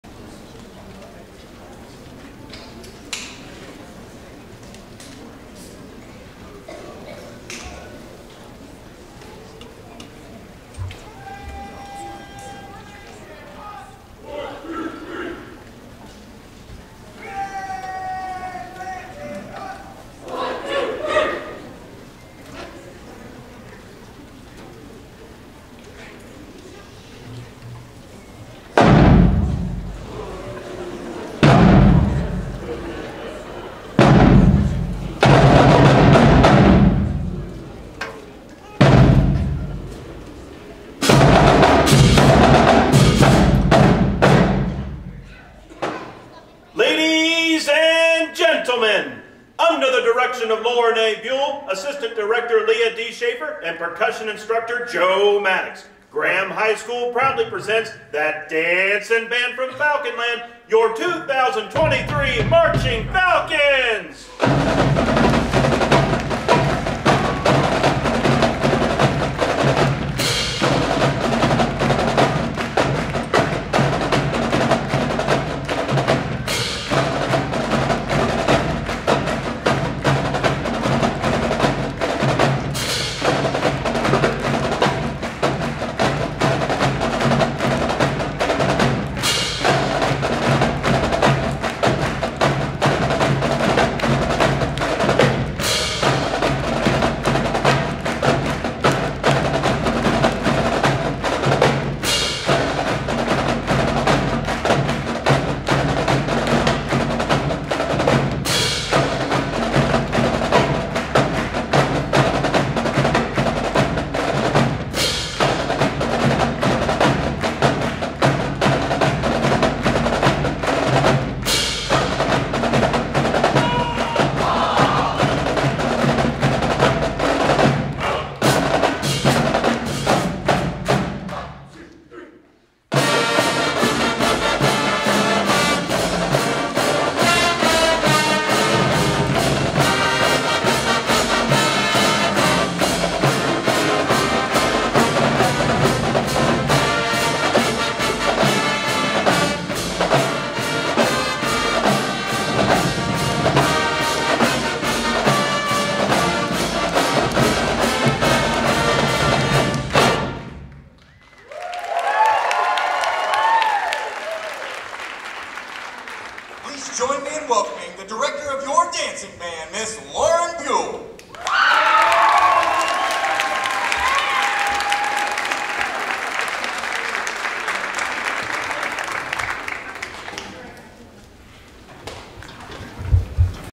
[4K] Drive-On Entrance & Fight Song - 2023 Graham HS "Dancin' Band" Varsity Show
Like the content?
November 4, 2023
Graham High School "Dancin' Band from Falconland"
Varsity Show indoor concert - Graham Middle School auditeria/cafetorium - St. Paris, Ohio